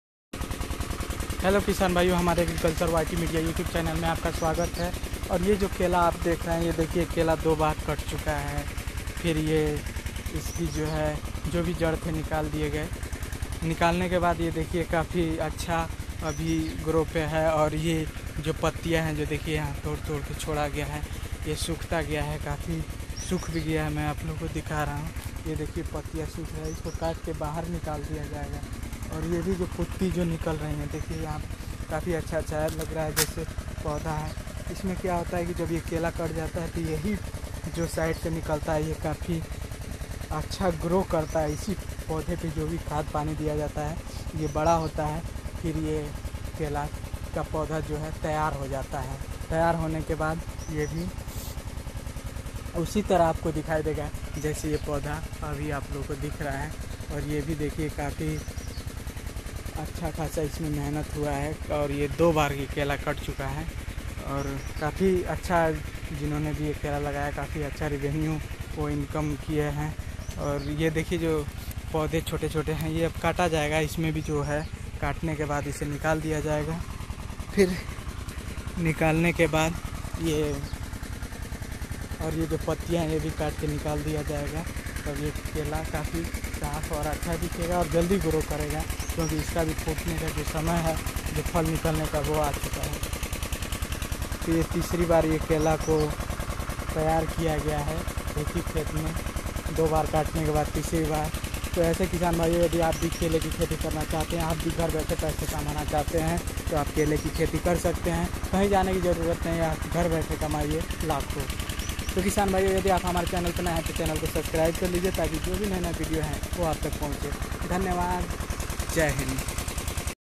लाखो का मुनाफा | एक बार केला लगाकर तीन साल तक की कमाई | Kele Ki Kheti Uttar Pradesh
सभी किसान भाइयों को नमस्कार ! Hello

MY GEARS :-

TRIPOD FOR MOBILE & CAMERAS